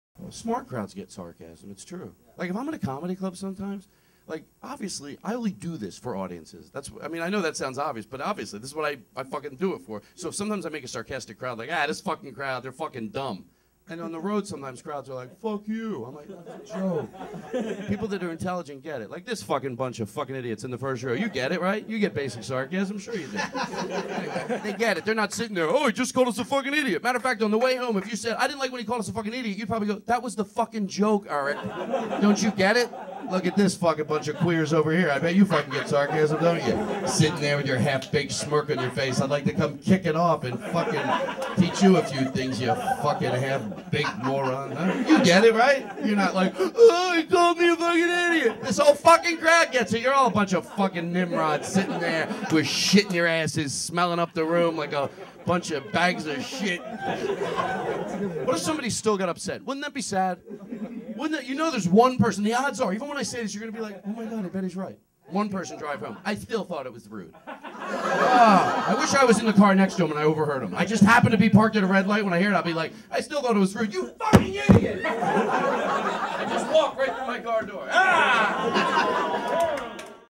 Stupid Comedy Crowds - Stand-Up Comedy - Todd Glass
Dumb crowds 'get it', right?